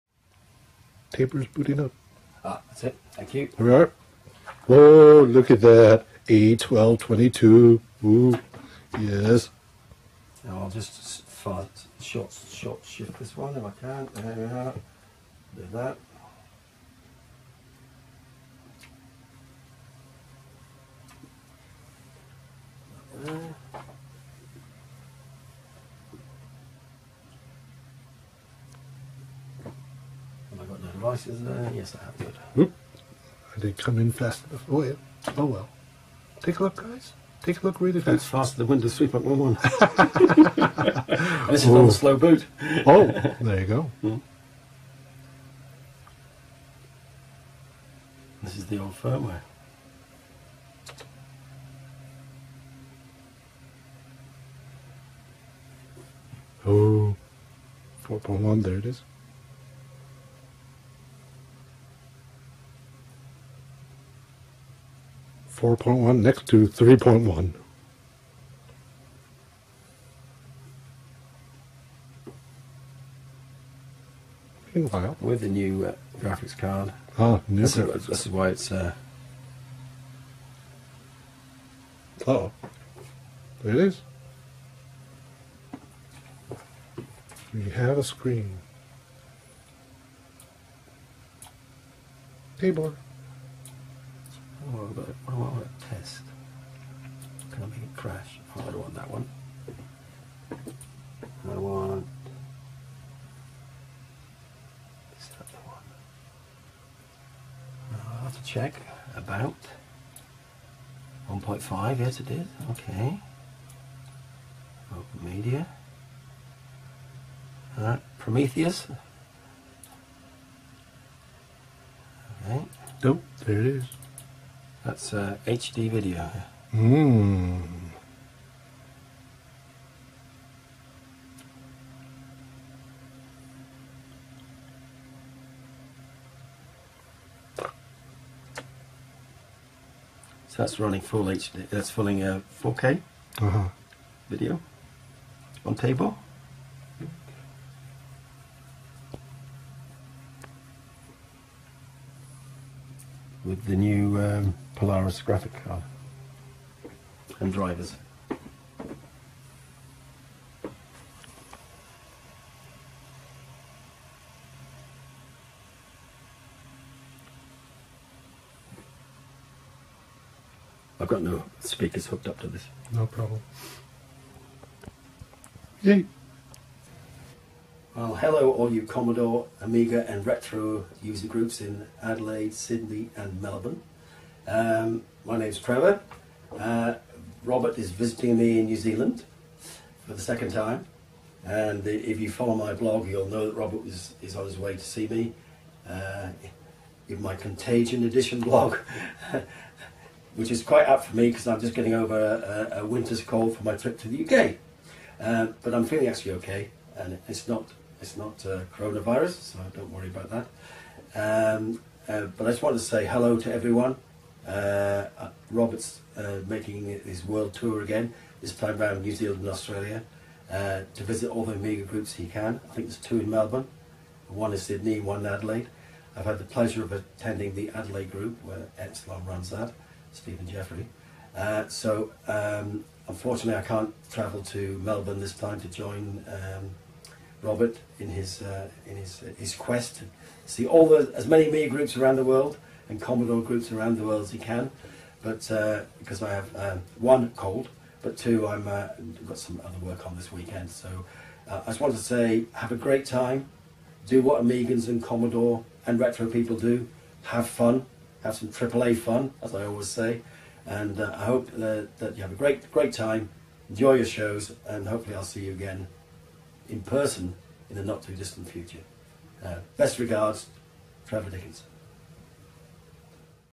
Booting an A1222 Tabor + Greetings from Trevor Dickinson
From his home workshop in New Zealand, Trevor Dickinson of A-EON boots up a prototype AmigaOne A1222 Tabor (without optimized start-up) and shows it displaying 4K video through its Sapphire video card.  Later in the video Trevor gives his greetings to the Commodore/Amiga clubs in Australia. (Note that the laptop in the lower part of the video is a Commodore PC Pentium laptop running Windows 3.1. It's video is

Filmed on Friday, February 28, 2020, by Bernardo